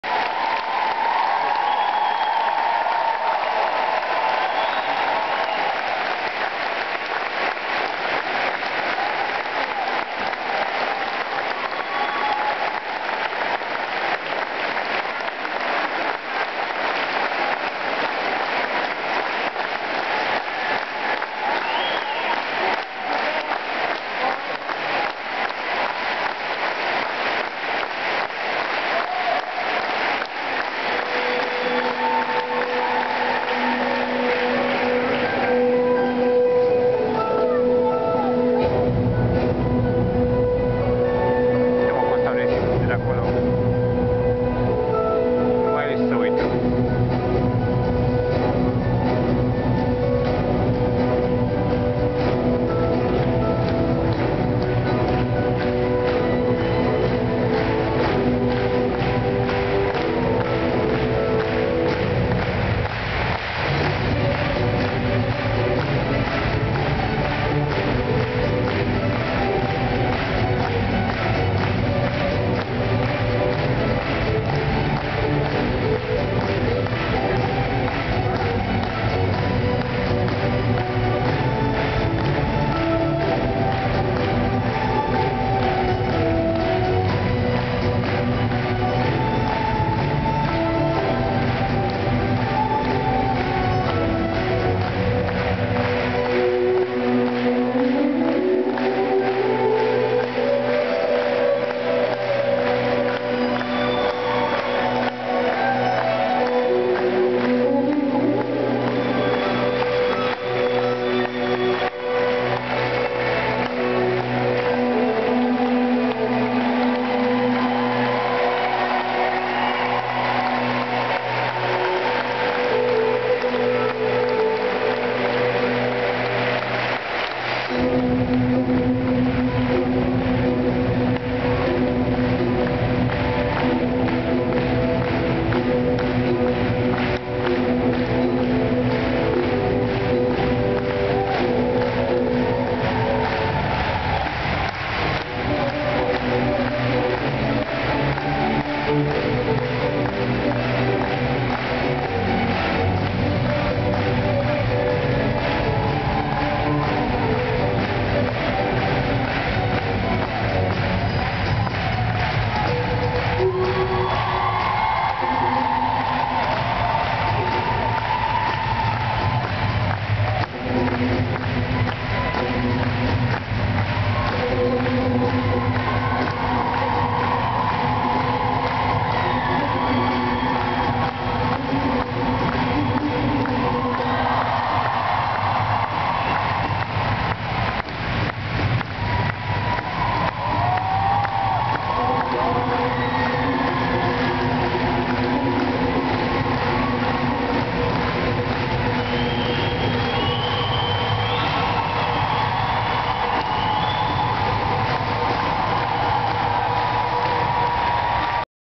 Final Kings on Ice - Bucuresti (07.04.2012) (filmare amator)
Final Kings on Ice - Bucuresti (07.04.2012) la patinoarul Flamaropol.